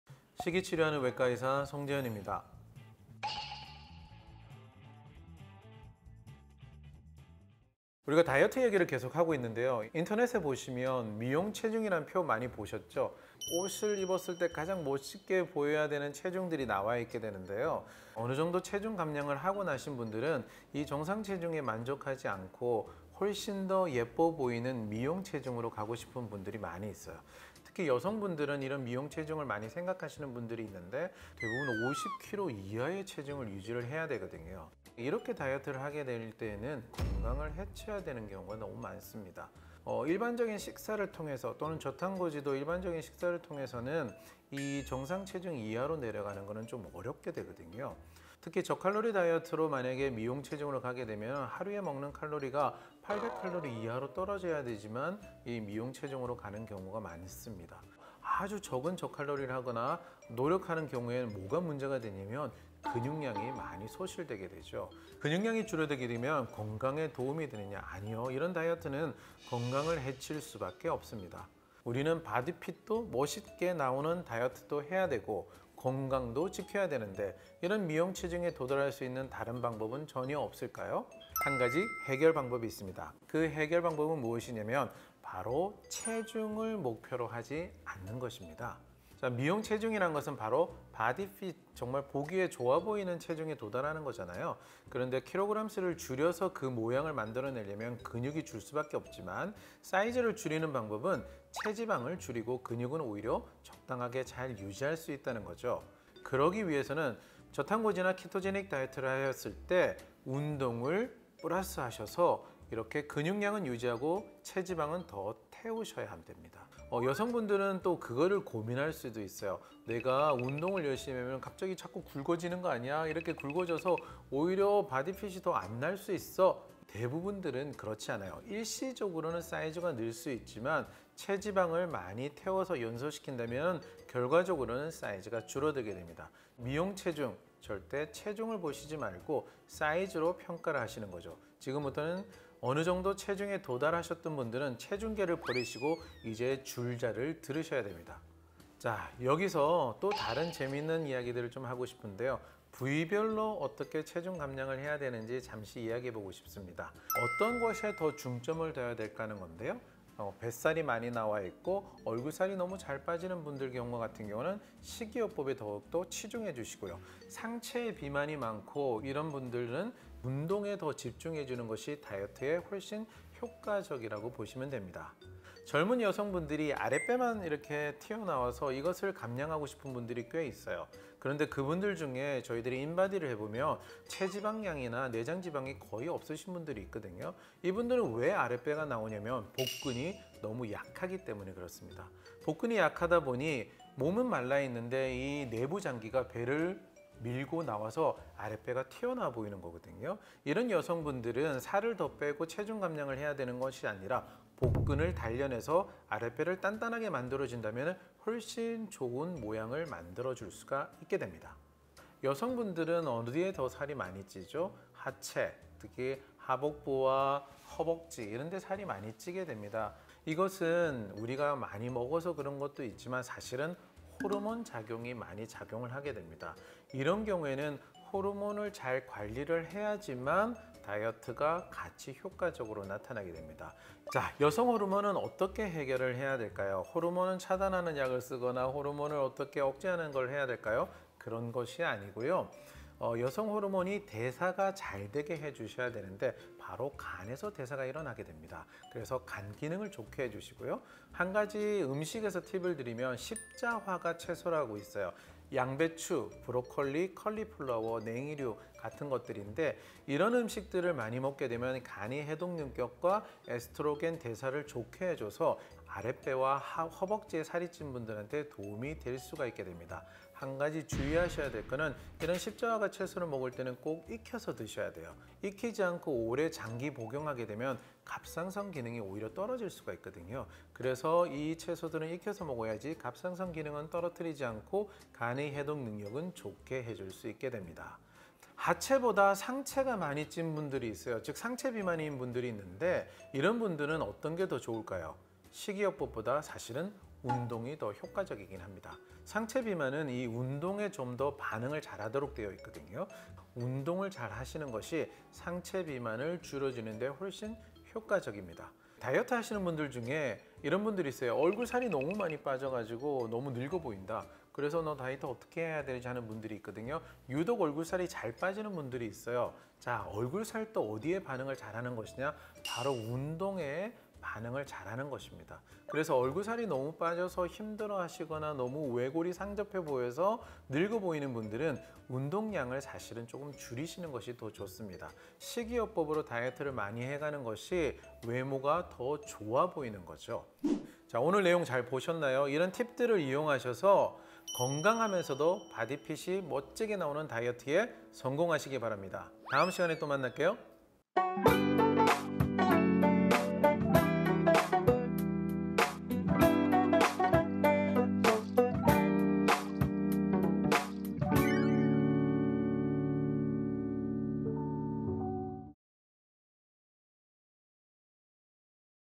나에게 맞는 부위별 다이어트로 모델 핏 만들기!
옷을 입었을 때 가장 예뻐 보인다는 미용 체중
하지만 터무니없이 낮은 목표 체중에 번번이 포기하게 되죠?
이번 영상은 미용 체중에 도달하지 않아도 모델 같은 옷 핏을 얻는 방법에 관해 이야기합니다!